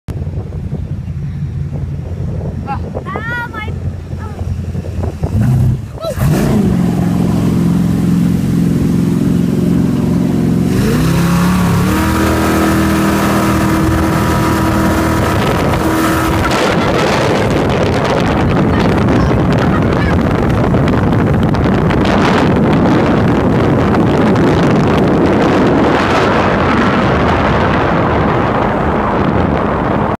Martin's jet boat
Taking the kids for a blast. Sydney November 2014
Martin, Elliot and Chloe